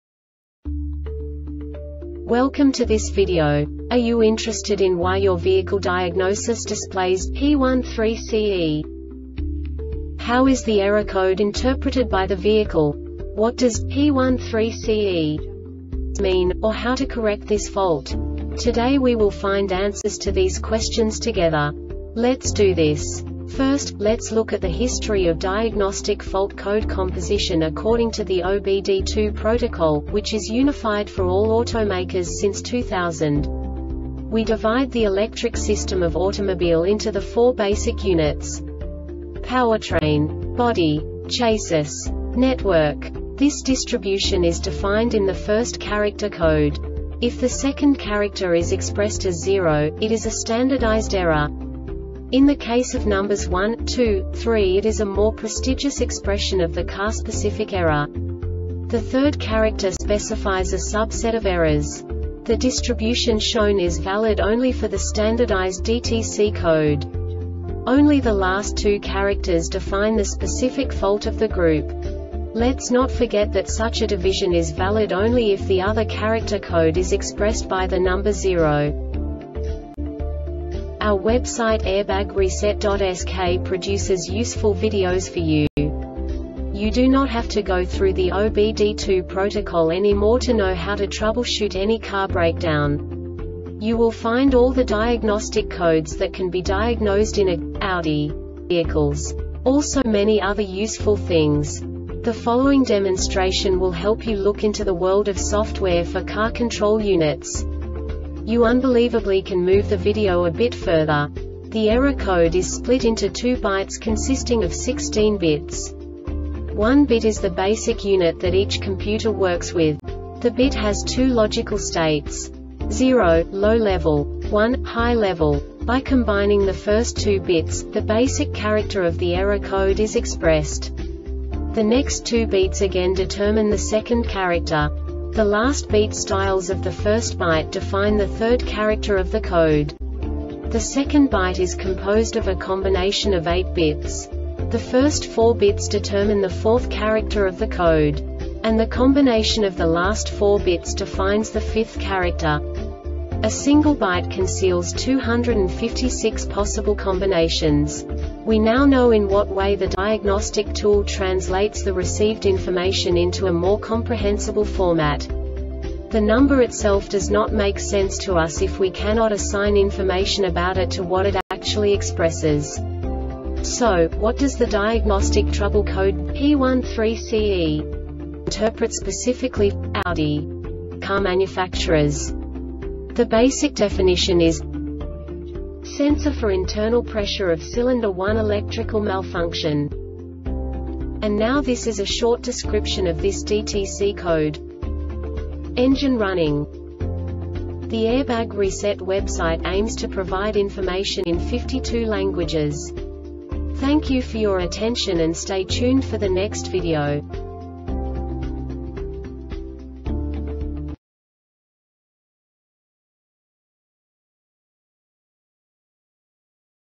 DTC Audi P13CE Short Explanation
Contents:
0:21 Basic DTC analysis according to OBD2 protocol standard.
1:48 Insight into programming
2:58 A diagnostically understandable description of the Diagnostic Trouble Code
3:05 Basic error definition

Make:
Audi
Code:
P13CE
Definition:
Sensor for internal pressure of cylinder 1 Electrical malfunction
Description:
Engine running